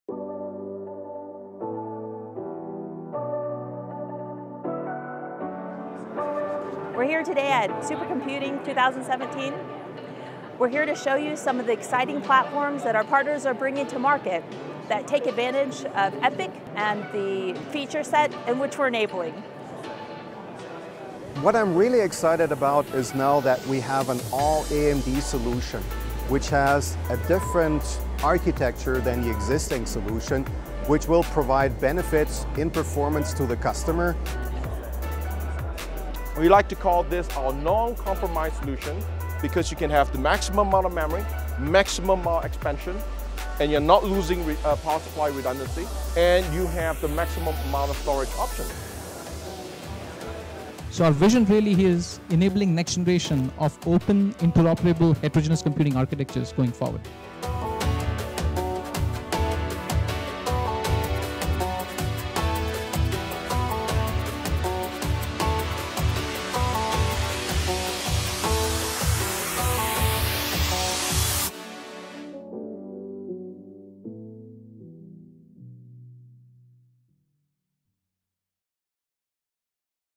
AMD Takes the Stage at SC17
This year at Supercomputing 17, AMD is presenting a full suite of high-performance computing systems spanning across CPU and GPU technologies, all supported by ROCm software. The spotlight is on ecosystem partners offering a range of server and datacenter platforms supporting AMD EPYC™ processors and AMD Radeon Instinct™ accelerators.